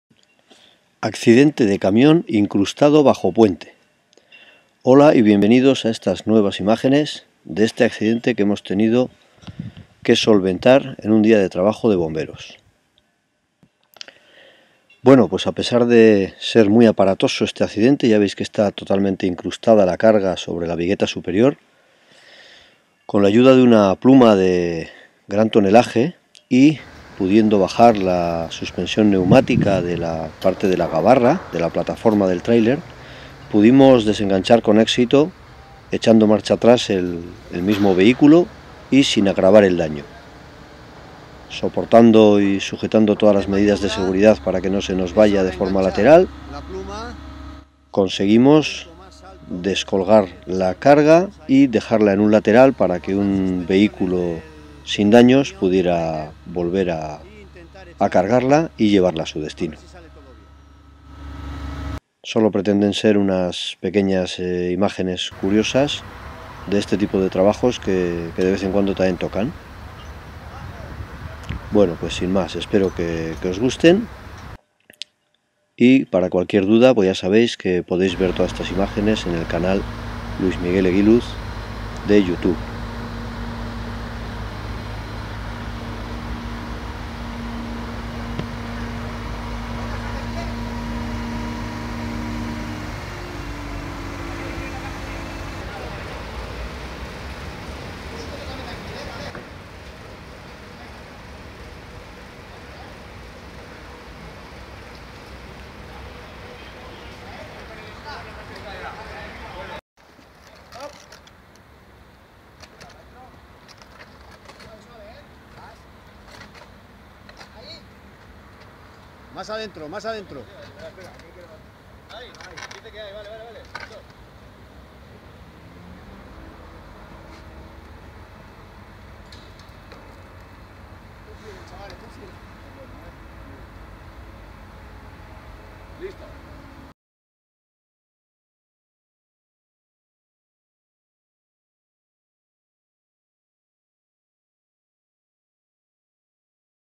Accidente de camion y su carga
Vemos como el trabajo de Bomberos es entretenido, variado y no exento de ciertos riesgos que debemos prevenir.